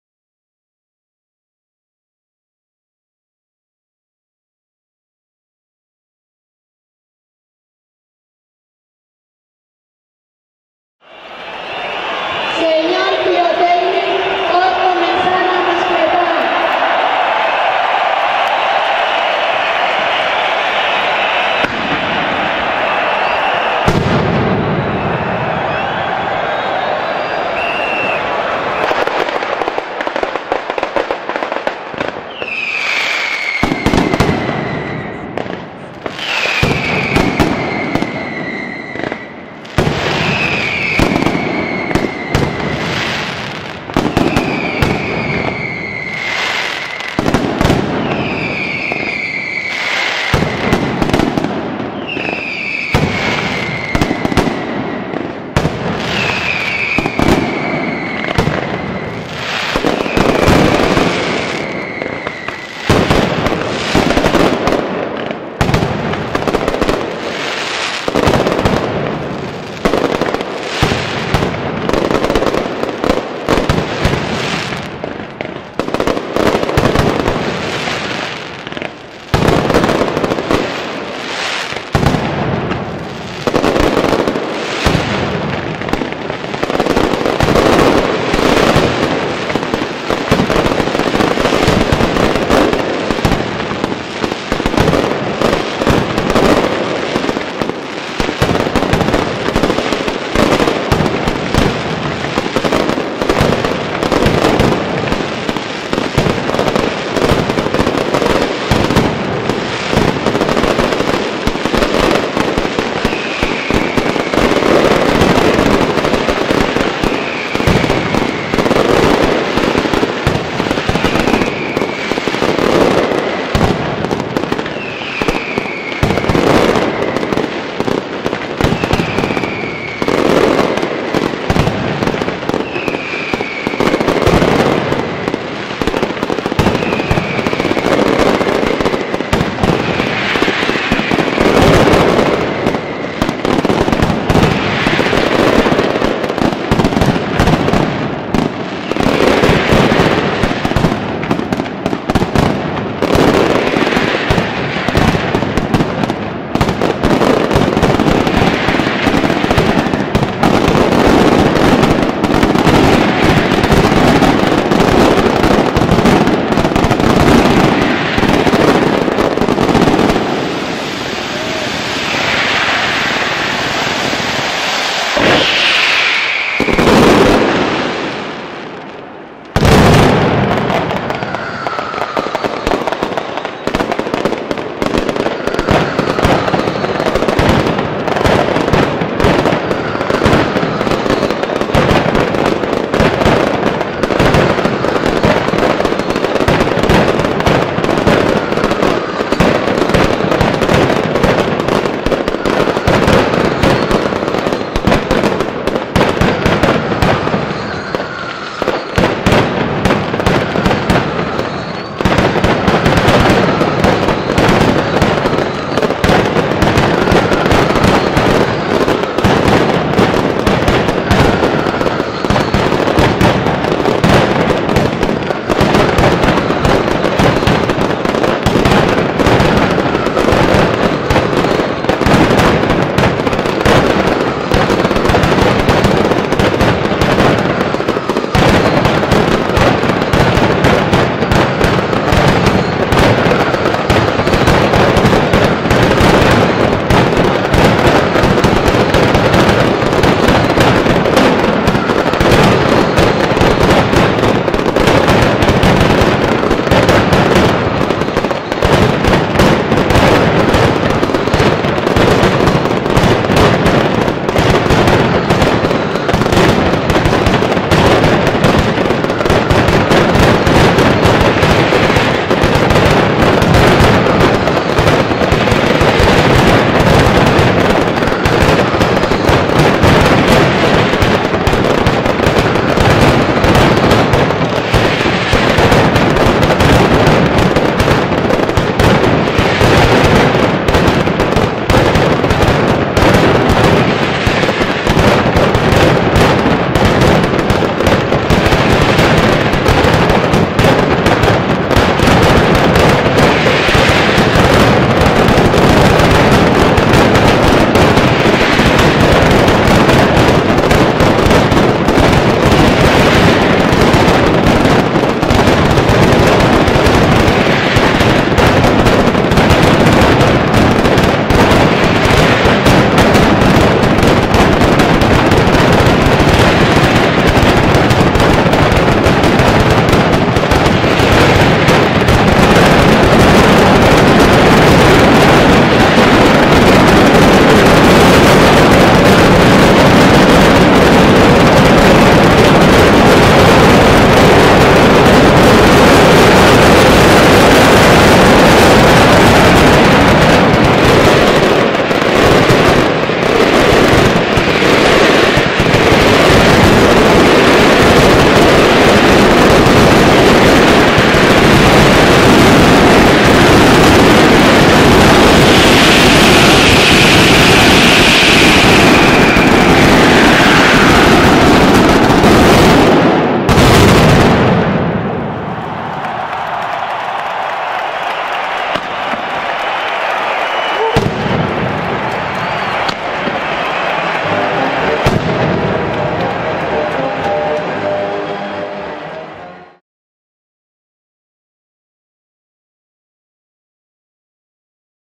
Fallas de Valencia 2012. Mascletà Pirotecnia Valenciana
Pirotecnia Valenciana. Mascletà de la plaza del Ayuntamiento. Fallas 2012. Dedicada y dando soporte a los afectados de Cancer. Las Fallas de Valencia 2012.